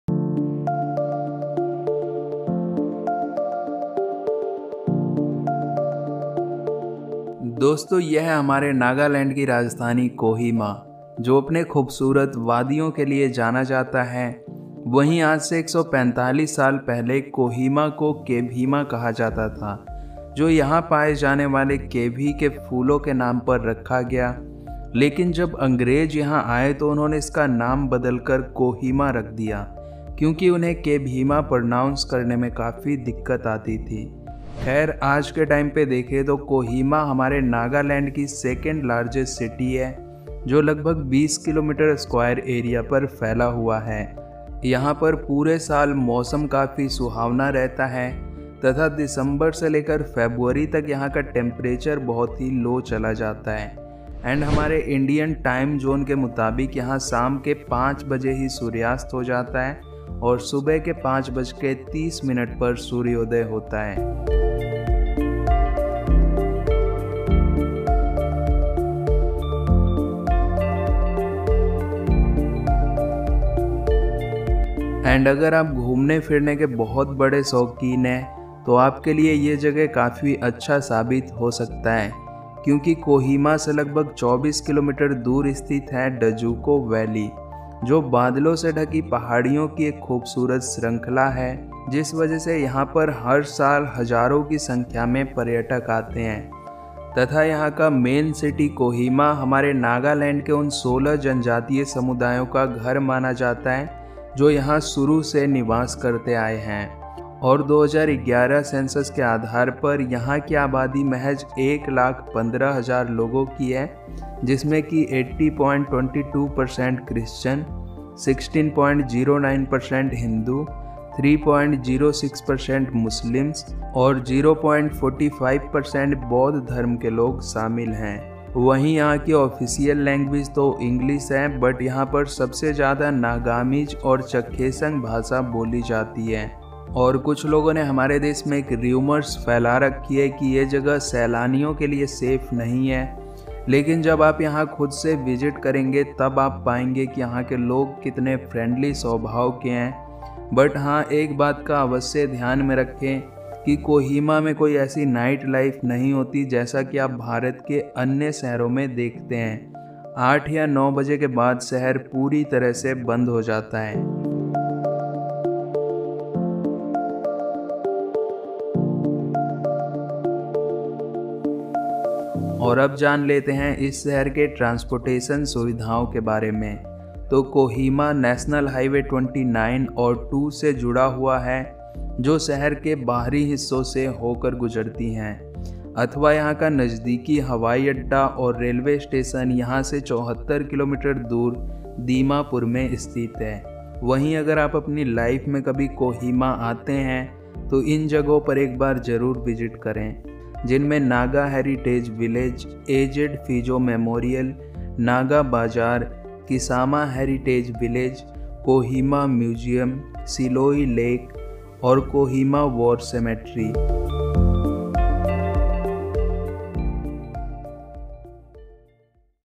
Kohima City | capital of Nagaland | Informative video 🌿🇮🇳
This video gives information about Kohima, the capital of the Indian state of Nagaland.In which we will know about the history, culture and tourist places of Kohima city.

---Top 7 best places to visit in Kohima---

(1) Naga Heritage Village

(2) A. Z. Phizo Memorial

(3) Shilloi Lake

(4) Nagaland State Museum

(5) Dzukou Valley

(6) Kohima war cemetery

(7) Naga bazzar